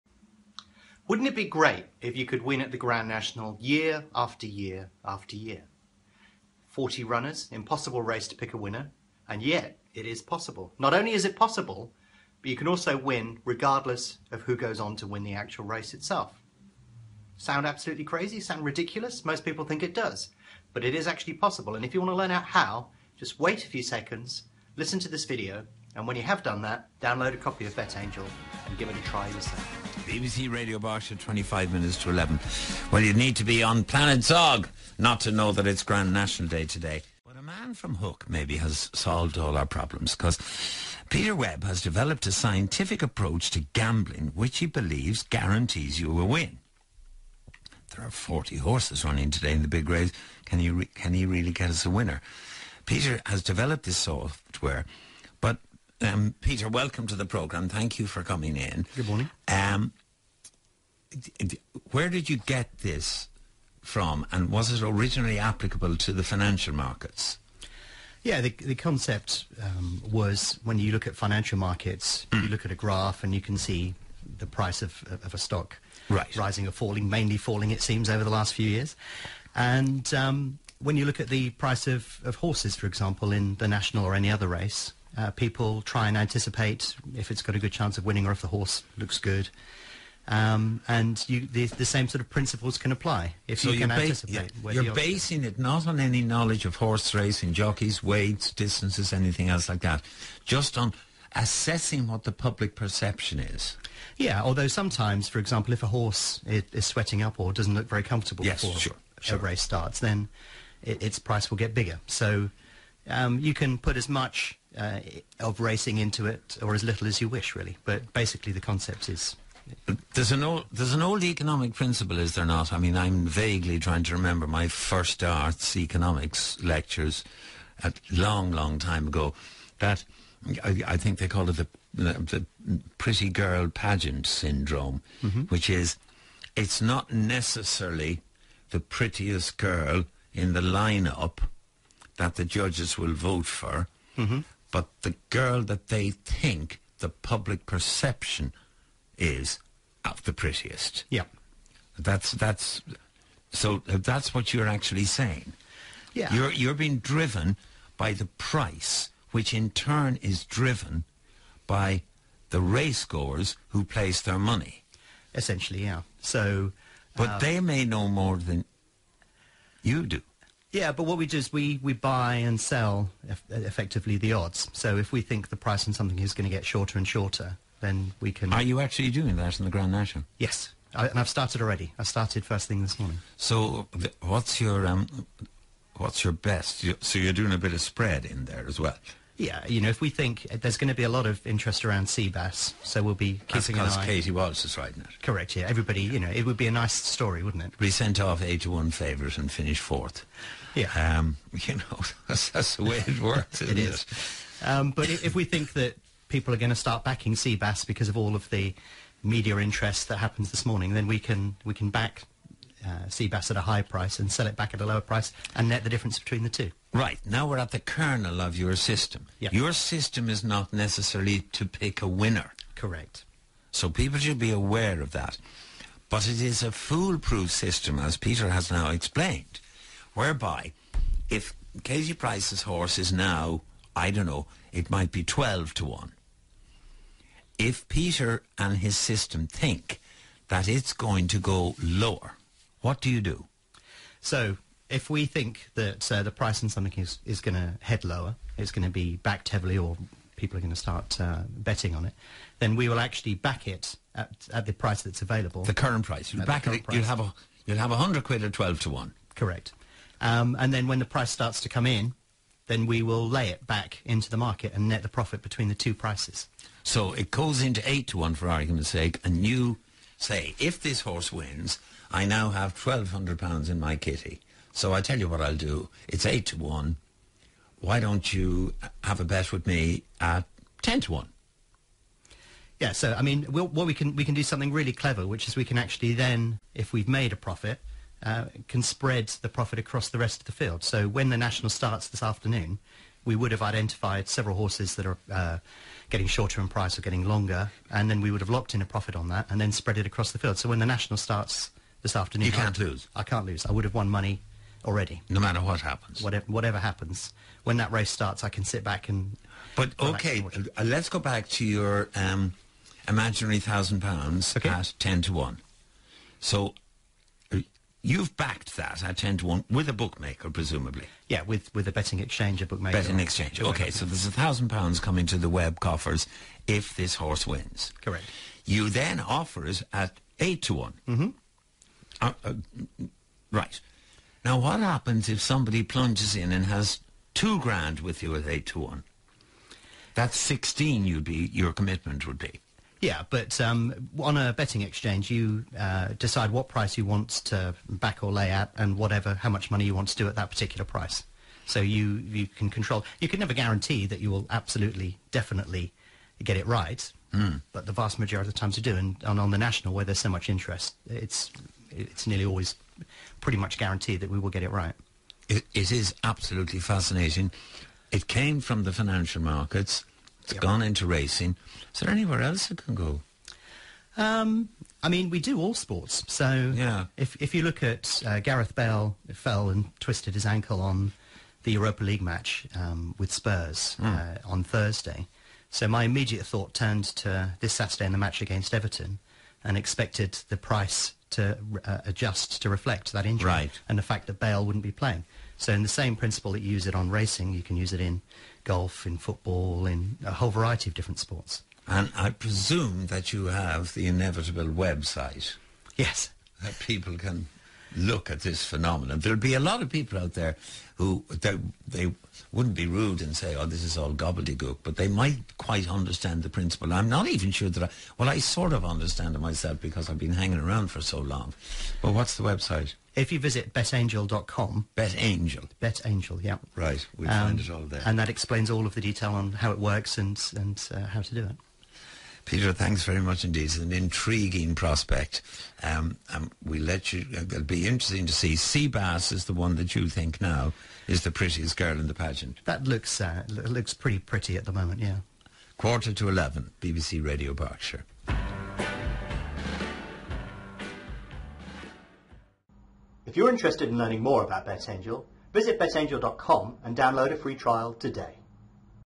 How to really win a bet on the Grand National - Peter Webb - BBC Interview
For a large number of years now, we have been pulling out some amazing results from betting markets.

With 40 runners, the Grand National is a notoriously difficult race to pick a winner. So the BBC were intrigued how I had managed to do that for a number of years.

They invited me in for an interview to discuss things. In the interview, I talk about Bet Angel, Betfair, Betfair trading and how I've managed to win whatever the result of the Grand National.

Betfair trading is similar to matched betting.